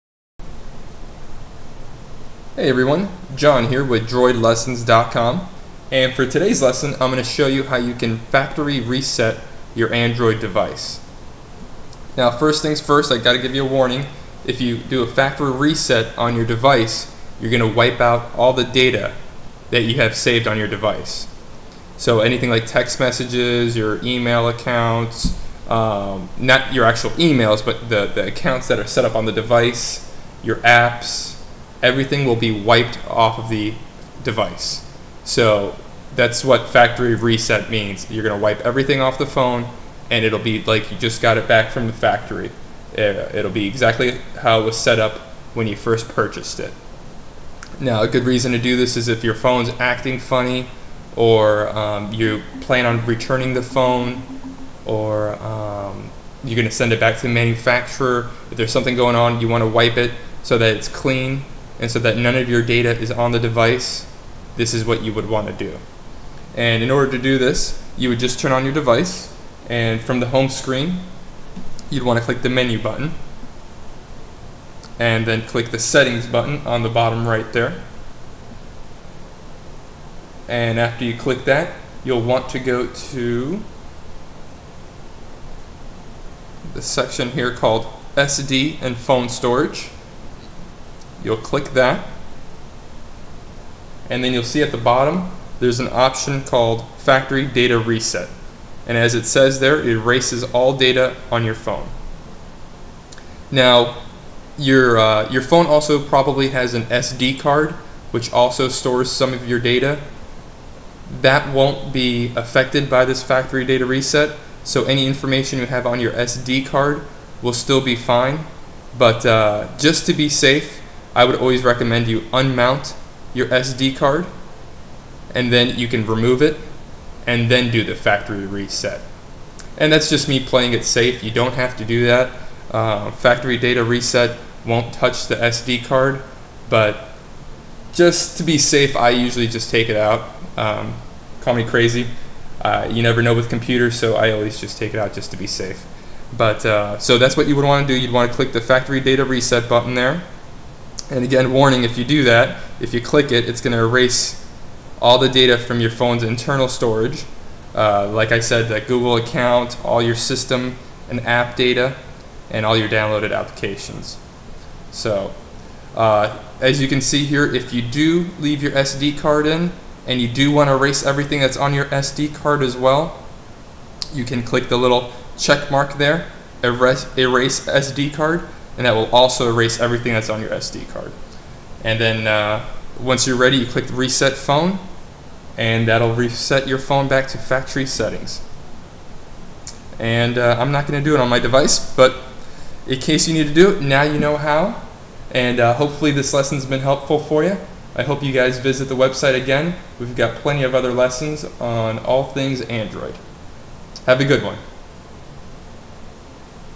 How to Factory Reset your Android smartphone
In this lesson I will show you how you can Factory Reset your Android device. WARNING: This will erase all your data on your phone!! Check out the full tutorial on factory resetting your Android on my